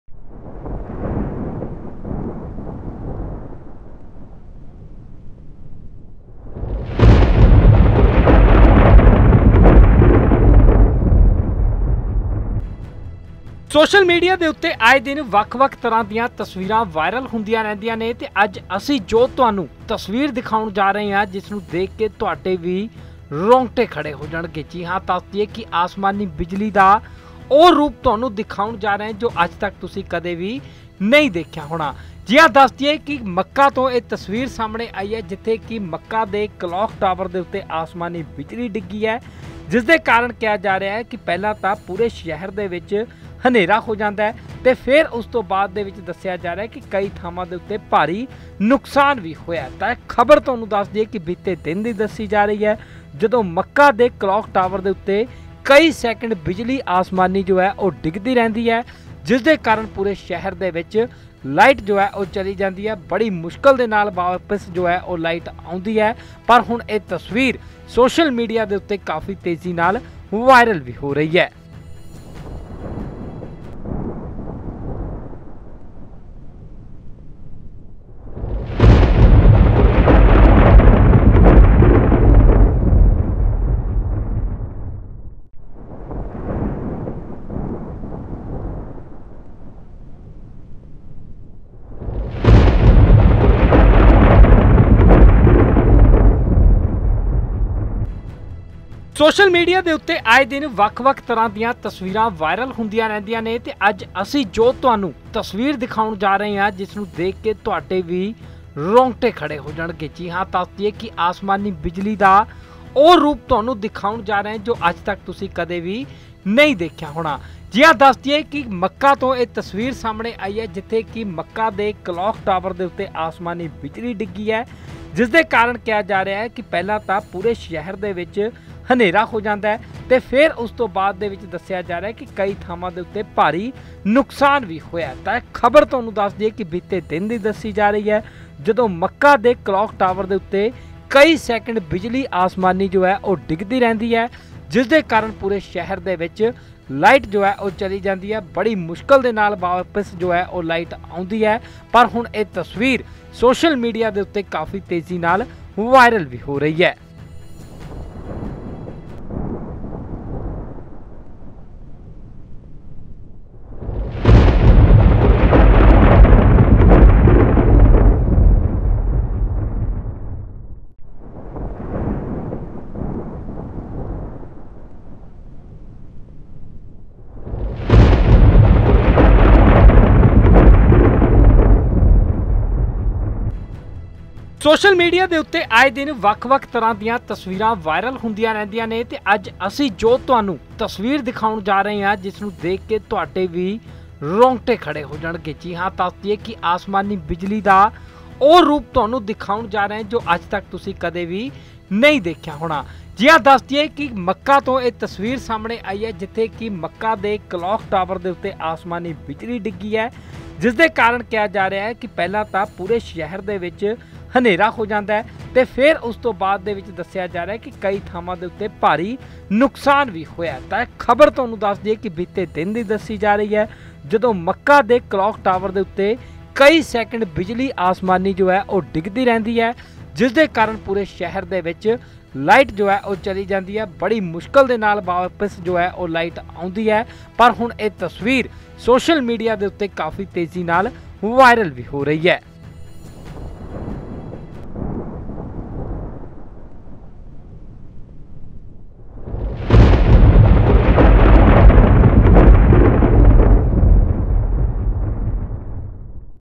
Mecca 'ਚ ਡਿੱਗੀ ਅਸਮਾਨੀ ਬਿਜਲੀ, ਕਲਾਕ ਟਾਵਰ ਨੂੰ ਬਣਾਇਆ ਨਿਸ਼ਾਨਾ | Lightning strikes Mecca Clock Tower | N18G
ਮੱਕਾ 'ਚ ਡਿੱਗੀ ਅਸਮਾਨੀ ਬਿਜਲੀ ਕਲਾਕ ਟਾਵਰ ਨੂੰ ਬਣਾਇਆ ਨਿਸ਼ਾਨਾ। ਦੇਖੋ ਆਸਮਾਨੀ ਬਿਜਲੀ ਦਾ ਵਿਰਾਟ ਰੂਪ, ਵੀਡੀਓ ਹੋਈ ਸੋਸ਼ਲ ਮੀਡੀਆ 'ਤੇ ਵਾਇਰਲ।